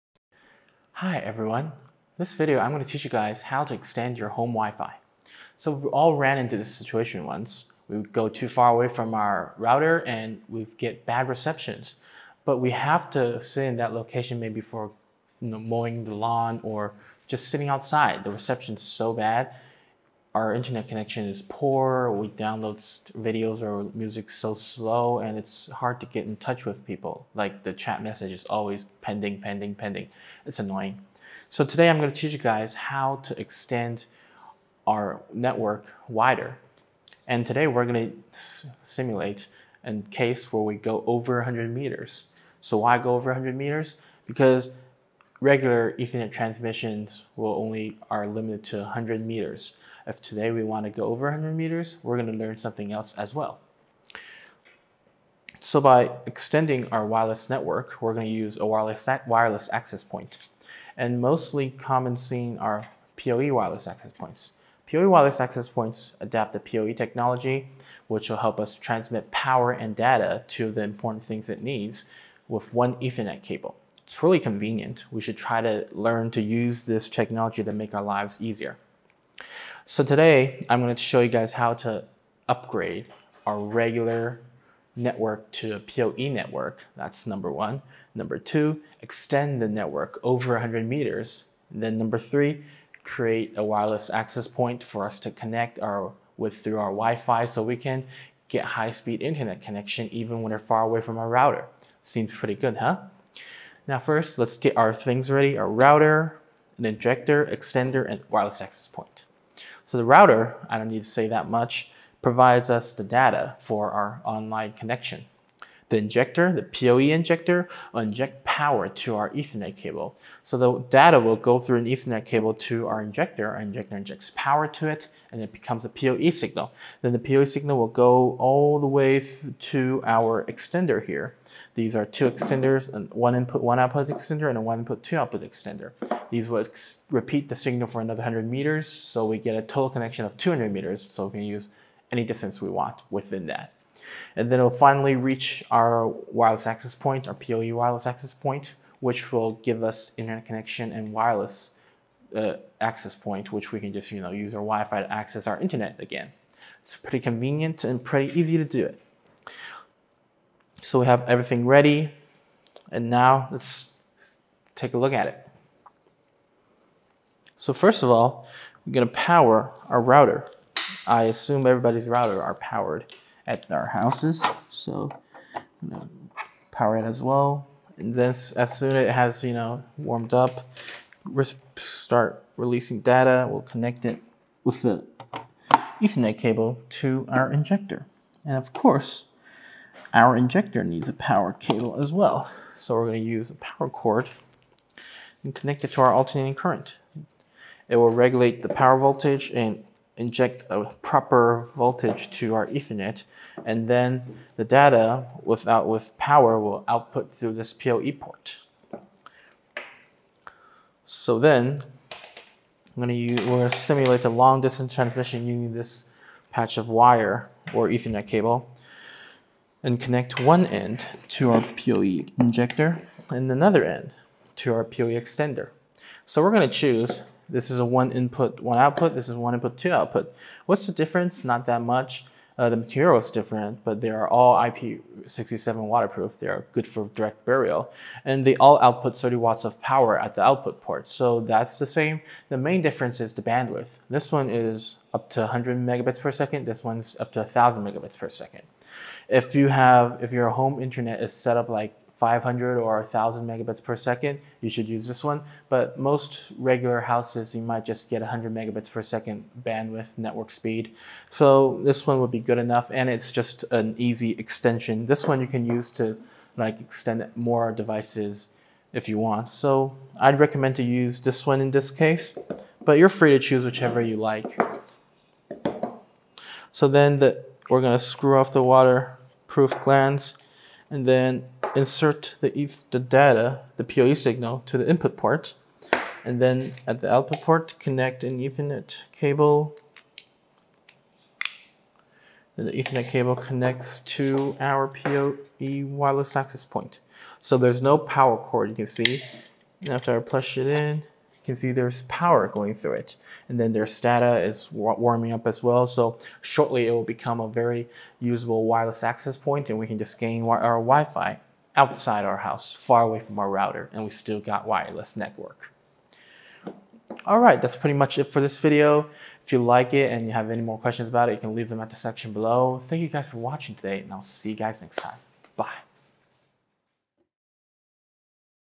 Using Single Device to Extend Your Home WIFI with Ethernet Cable
This video teaches us how to extend our home Wi-Fi with an Ethernet cable. We all have run into those moments when we can’t get a good reception outside of our house, if we extend our home Wi-Fi, we will be able to get great access even far away from our router. In this case, we would use a POE wireless access point. A device that can set up an access point for us to connect to the internet.